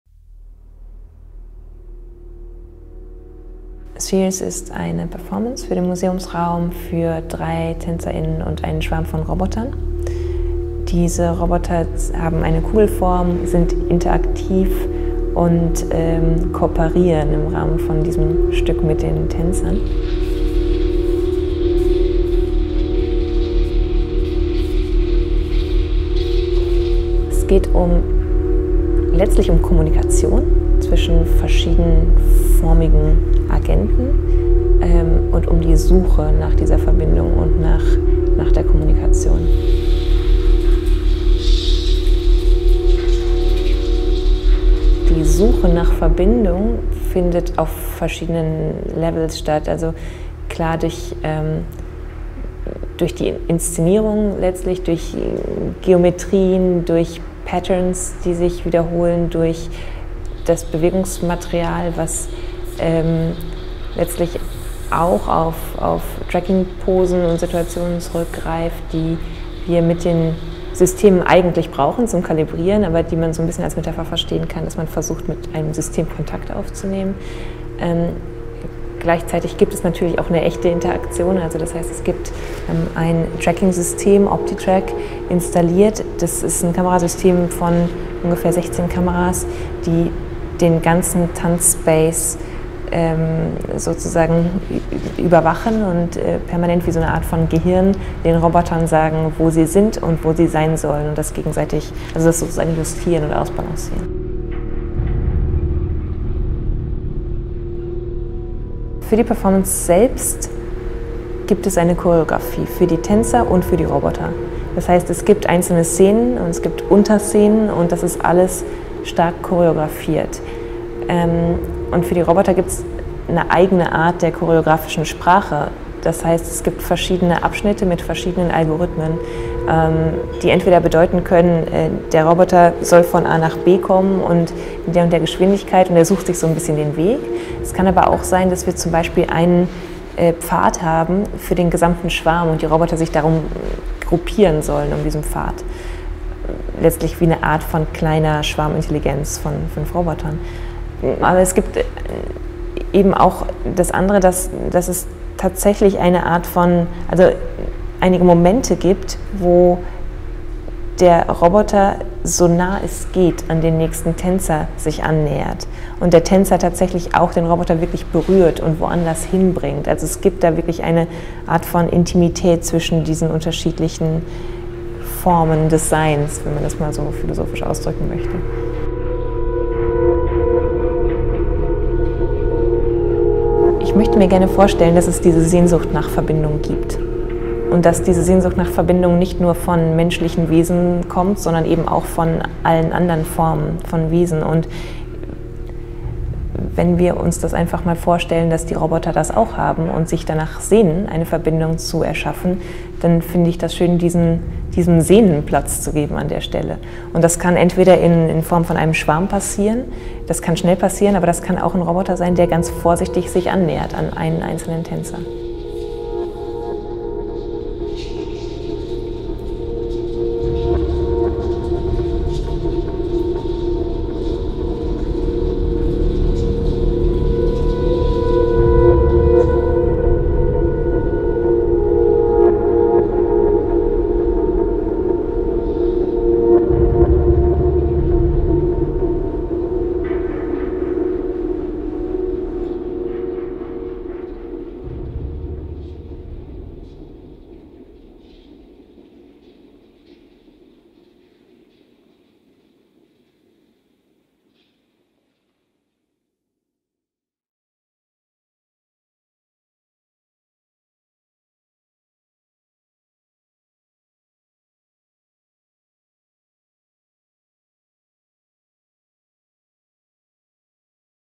Spheres – Featurette – Charlotte Triebus
Choreography, Concept, Lead
Charlotte Triebus

Dancers
Aaron Samuel Davis
Esther Siddiquie
Maayan Reiter
Yurika Sophie Yamamoto
Juan Corres Benito

Dramaturgy
Reut Shemesh

Production
Mirevi Lab (HSD)

Project Management
Charlotte Triebus

Scientific Consultation And Technical Coordination
Christian Geiger

Software Development
Naoto Hieda

Technical Lead
Alexander Giesbrecht
Christoph Vogel

Technical Implementation
Daniel Trabitzsch
Fabian Büntig
Sven Heinen

Hardware Development
Tennagels Medientechnik, Marco Strobel, MIREVI (HSD)

Previous Research & Development
Neoanalog, Michael Schmitz

Financial Management
Ivana Druzetic

Video Production
Klangmalerei.tv

Light
Kristof Schloesser

Assistance
Jonas Becker

Spheres is a joint project of MIREVI (HSD) and Charlotte Triebus, funded by Kunststiftung NRW, E.ON Stiftung, BMBF (Gina), the European Regional Development Fund and the Ministry of Economic Affairs, Innovation, Digitization and Energy of NRW (MARTA, Distel), Tennagels Medientechnik and NRW Forum.